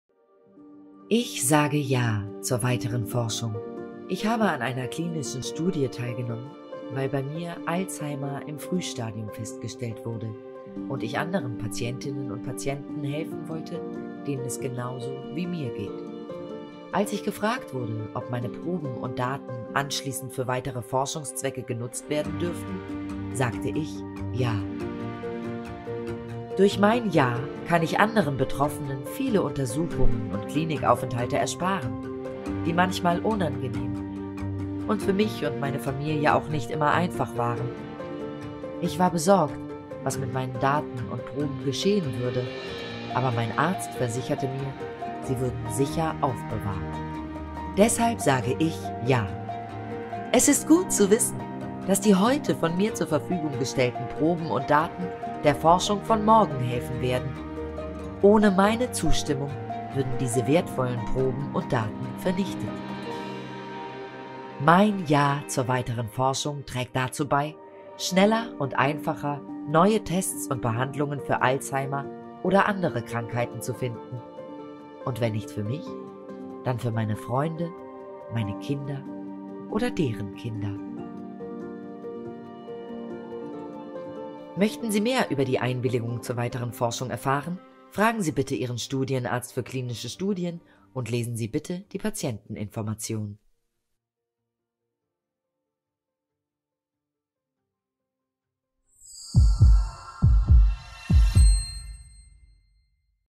FUMO (WOGT) - Sagen Sie JA zur weiteren Forschung
Wenn Sie an einer klinischen Studie teilnehmen, werden Sie um die Erlaubnis gebeten, Ihre Proben und Daten für weitere Forschungszwecke freizugeben. Wenn Sie der weiteren Forschung zustimmen, können Sie anderen Menschen viele Tests und Klinikbesuche ersparen.Die Proben und Daten werden sicher aufbewahrt.

Indem Sie der weiteren Forschung zustimmen, können Sie dazu beitragen, dass neue diagnostische Tests und Behandlungsmöglichkeiten schneller und einfacher gefunden werden. Sehen Sie sich dieses Video an, um mehr darüber zu erfahren.